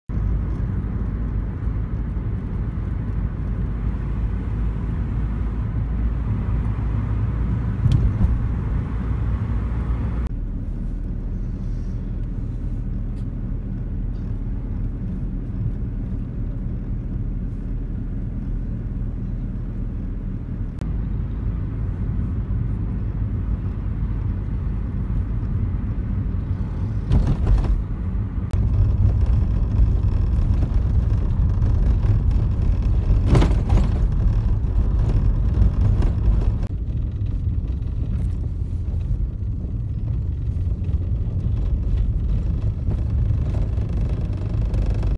Report TV -Fierakët thuajse të gjithë brenda! 'Kyçen' në shtëpi pas urdhrit të Ramës
Report Tv ka bërë një vëzhgim edhe në qytetin e Fierit, ku në rrugë ka vetëm një numër të vogël njerëzish, teksa pjesa dërrmuese i janë bindur urdhrit.